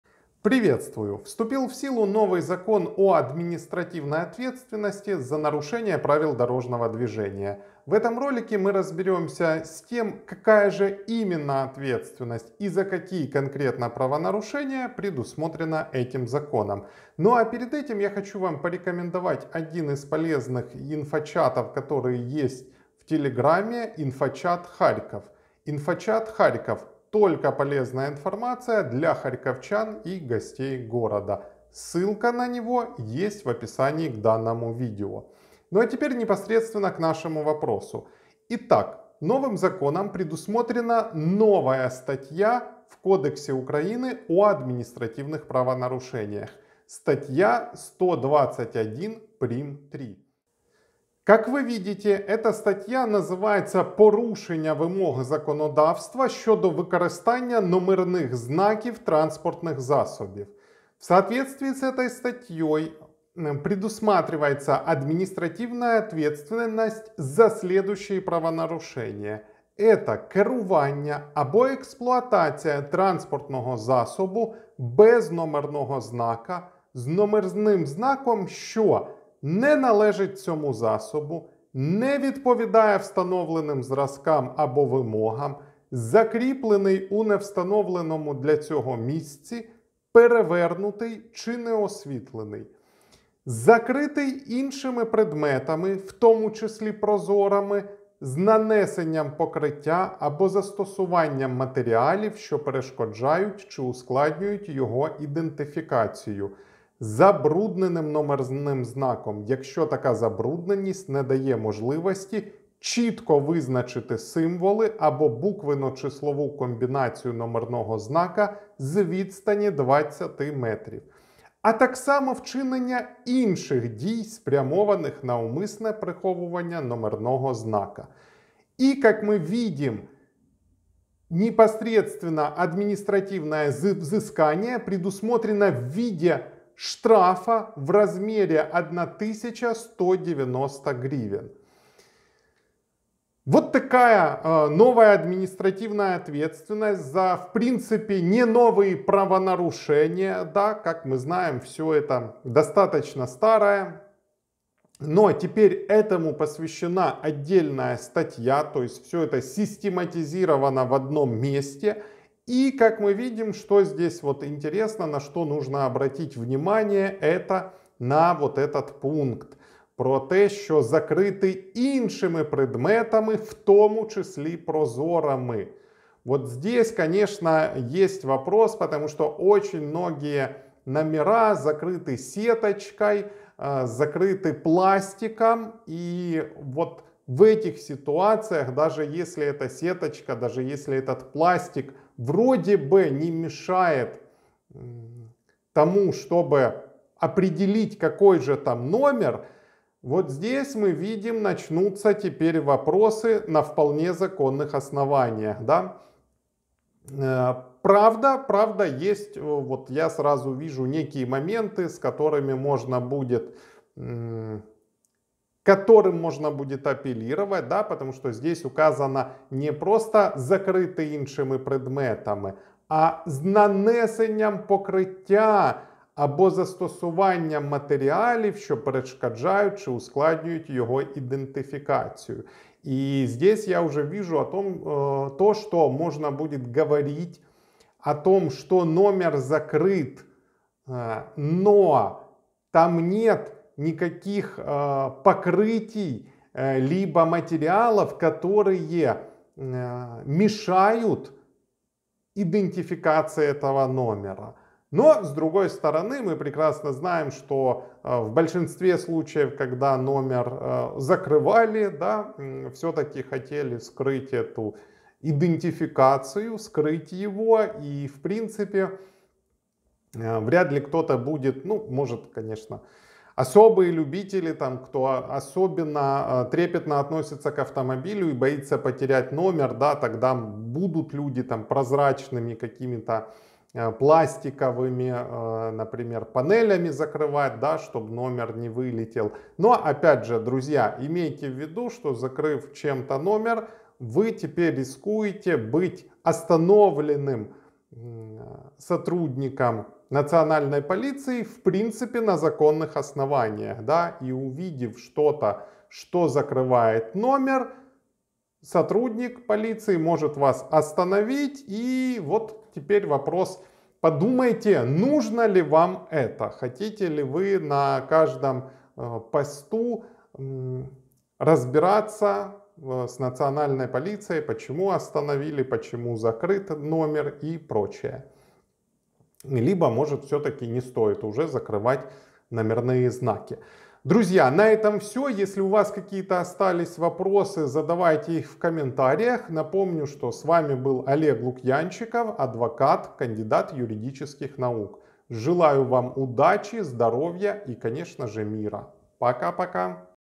Новый штраф ПДД Украина
Вступил в силу новый закон о штрафах за нарушение ПДД. В Кодексе Украины о административных правонарушениях появилась статья 121-3, которой предусмотрена ответственность в виде штрафа за нарушение правил использования номерных знаков автомобиля/мотоцикла.